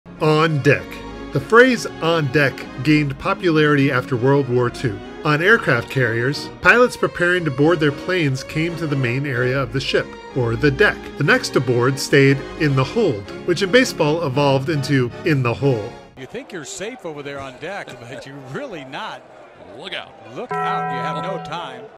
What Does "On Deck" Mean in Baseball?
In this I show off the 10th card in the Topps 2022 Allen & Ginter Chrome mini from the Baseball Lexicon insert set. What is the meaning of the baseball term, "On Deck"?

On Deck

The term "Frozen Rope" can refer to a throw from the outfield or a line drive off the bat. In both cases, the ball has to have plenty of "mustard" - so much that it appears to have no vertical drop over a long distance.  Like a rope would if it were iced over.

* Words from Topps 2022 Allen & Ginter Chrome Baseball Lexicon BL-10 card.

Union of Trading Card and Comic Book collectors and content creators of YouTube